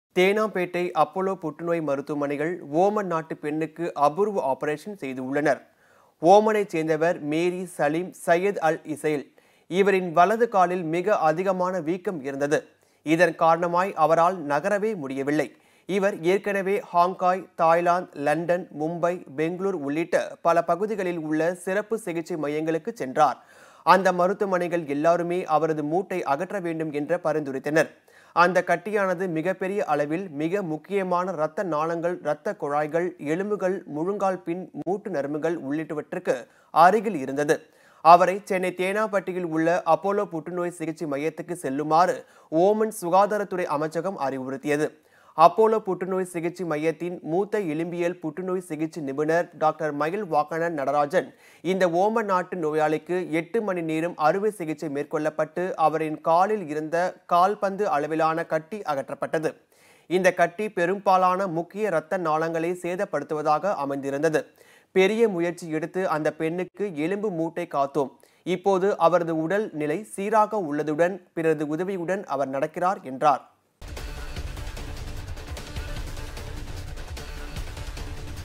தேனாம்பேட்டை அப்பல்லோ புற்று நோய் மருத்துவமனையில் ஓமன் நாட்டு பெண்ணுக்கு அபூர்வ ஆபரேஷன்
தேனாம்பேட்டை அப்பல்லோ புற்று நோய் மருத்துவமனையில் ஓமன் நாட்டு பெண்ணுக்கு அபூர்வ ஆபரேஷன் செய்துள்ளனர்.